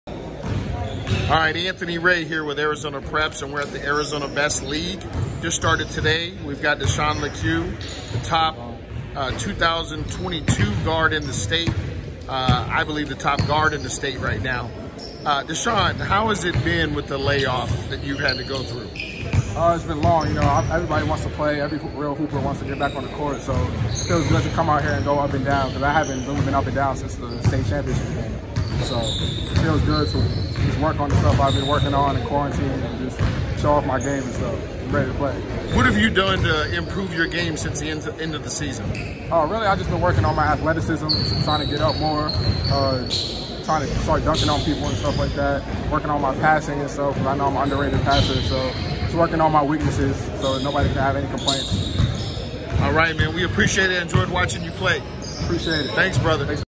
Arizona Preps Interview with Desert Vista High School’s Desean Lecque
Arizona Preps interviews top-rated 2022 guard prospect Desean Lecque regarding his recruitment and progress as a player.